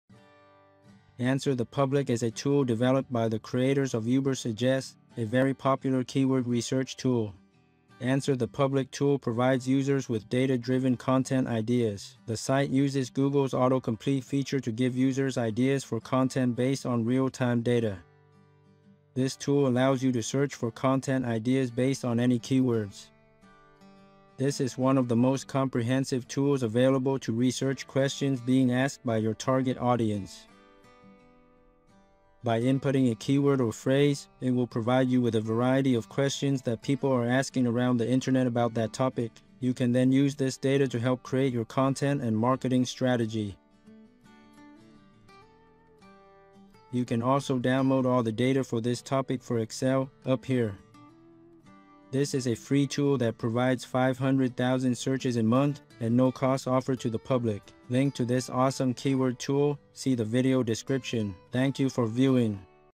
Free Keyword and Idea Research Tool For Content Creators - AnswerThePublic Review
AnswerThePublic is a free tool that allows you to search for content ideas based on any keywords. This is one of the most comprehensive tools available to research questions being asked by your target audience.

Try it and see for yourself.
*This tool is free but limited to 500k searches a month for the whole public.